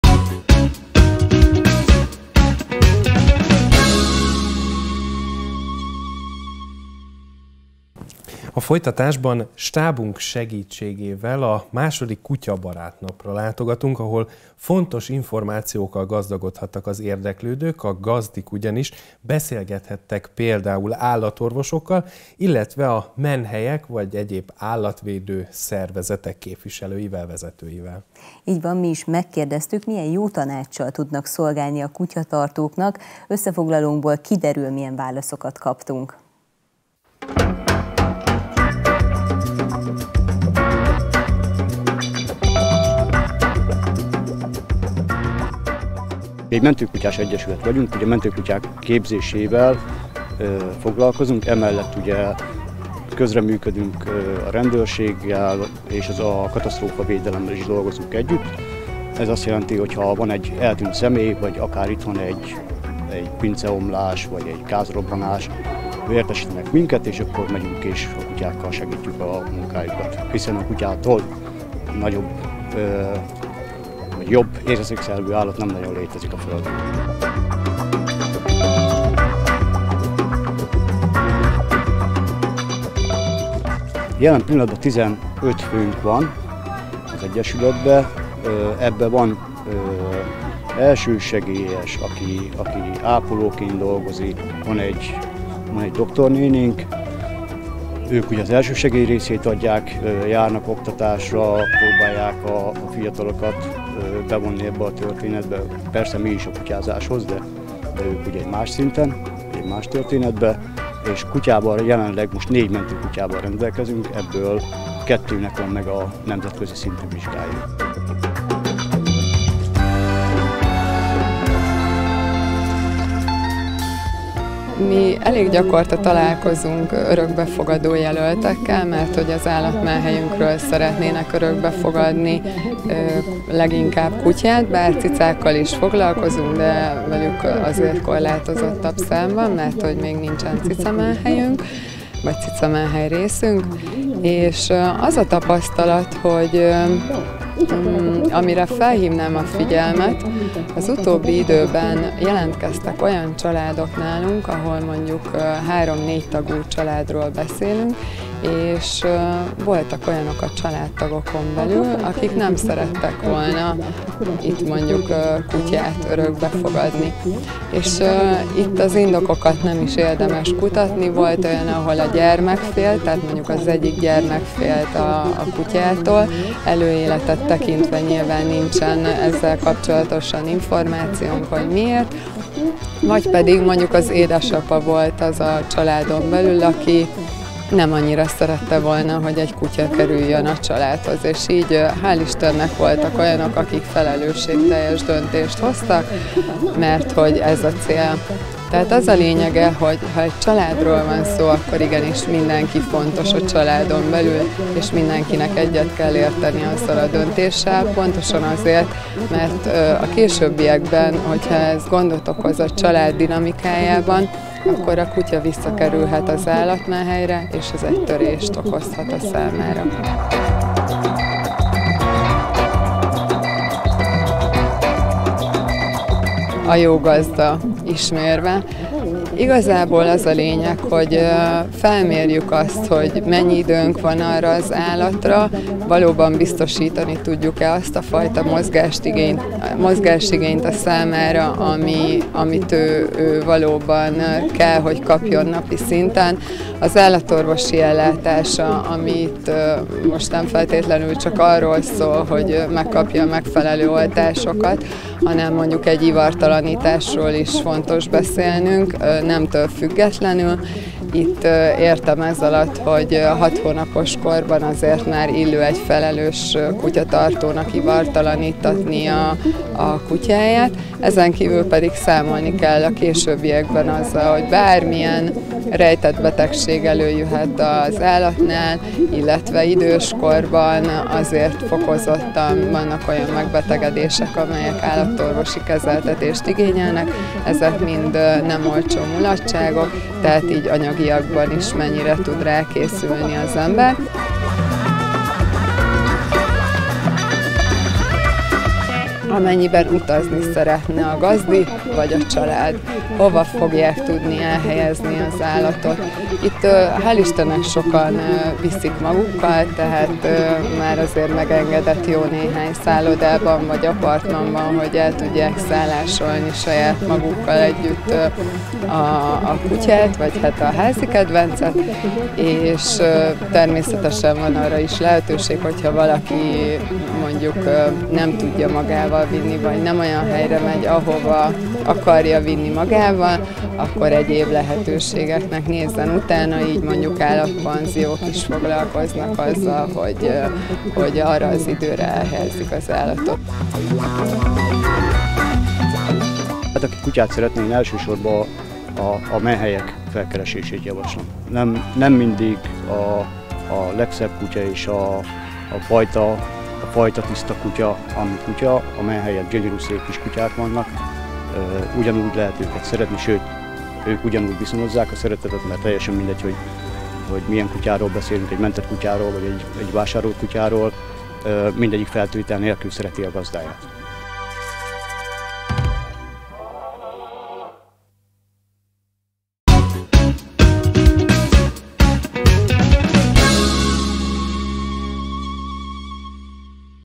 II. Egri Kutyabarát Nap – A felelős állattartást hirdették - 2023.06.27.
A II. kutyabarát napon alkalom nyílt arra, hogy a gazdik és a leendő gazdik fontos információkkal gazdagodjanak. Lehetőség nyílt például beszélgetni állatorvosokkal, menhelyek, és szervezetek vezetőivel. Mi is megkérdeztük, milyen jó tanáccsal tudnak szolgálnak a kutyatartóknak.